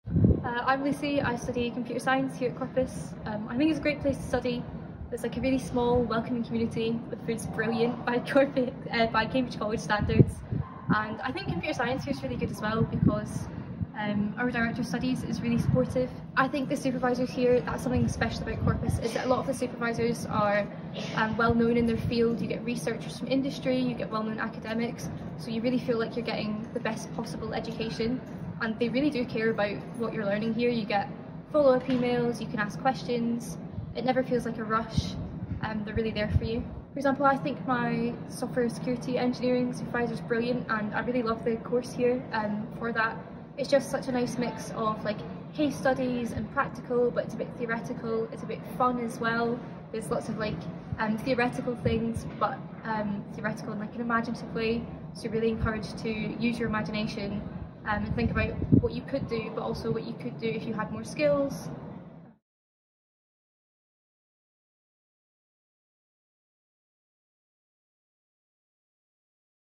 Computer Science at Corpus: a student view
Lucy, a First Year Computer Science student at Corpus Christi College, explains what it's like to study Computer Science at the University of Cambridge.